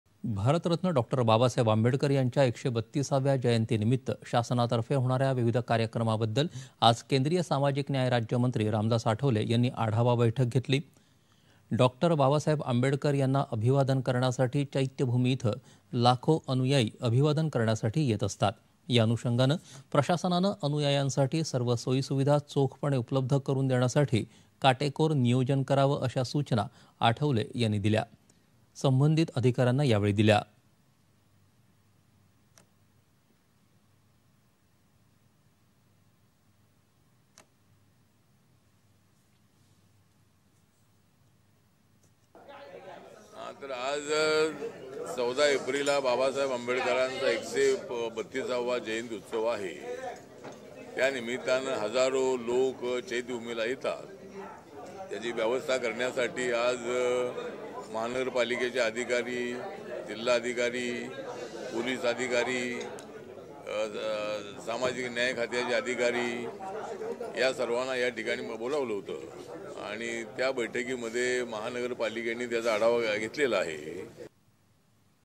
डॉ बाबासाहेब आंबेडकर यांच्या १३२ व्या जयंतीनिमित्त आढावा बैठक
दि.०२.०३.२०२३ :
 भारतरत्न डॉ बाबासाहेब आंबेडकर यांच्या १३२ व्या जयंतीनिमित्त शासना तर्फे होणाऱ्या विविध कार्यक्रमाबद्दल आज केंद्रीय सामाजिक न्याय राज्यमंत्री रामदास आठवले यांनी आढावा बैठक  घेतली.
डॉ बाबासाहेब आंबेडकर  यांना अभिवादन करण्यासाठी चैत्यभूमी इथं लाखो अनुयायी अभिवादन करण्यासाठी येत असतात.या अनुषंगानं प्रशासनानं  अनुयायांसाठी सर्व सोयी सुविधा चोखपणे उपलब्ध करून देण्यासाठी काटेकोरपणे नियोजन करावे, अशा सूचना  आठवले यांनी यावेळी दिल्या. संबंधित अधिकाऱ्यांना यावेळी दिल्या.
-- सिंक---.
रामदास आठवले,  केंद्रीय सामाजिक न्याय राज्यमंत्री.